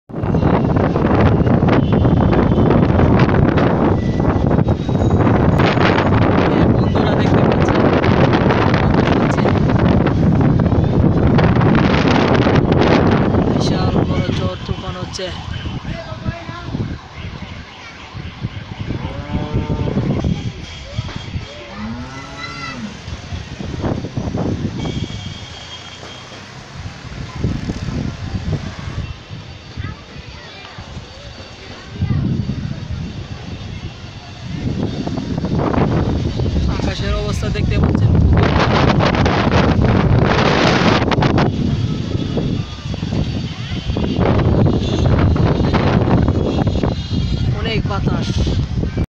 ভিডিও করতে গিয়ে আর একটুর জন্ন বাতাসে পড়ে যাচ্ছিলাম😀😀
জড় বাতাস